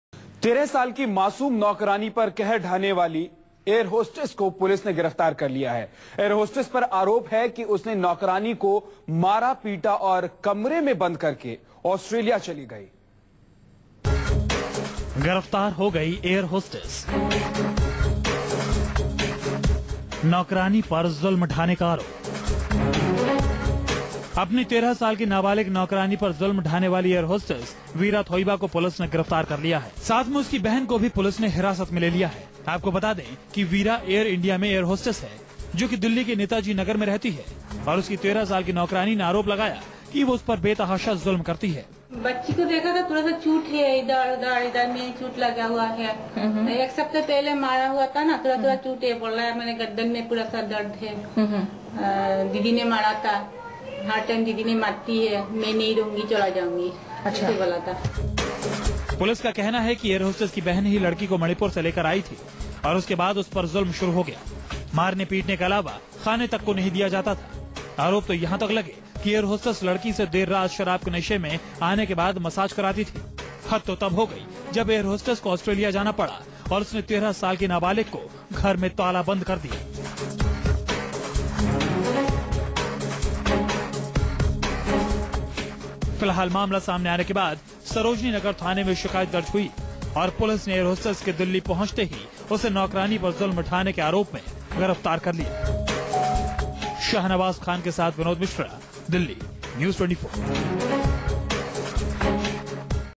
Delhi Air hostess who tortured 13 year old maid sent to judicial
An airhostess, arrested for allegedly assaulting her 13-year-old domestic help and locking her inside the house while she was abroad, was today sent to 14-day judicial custody by a Delhi court.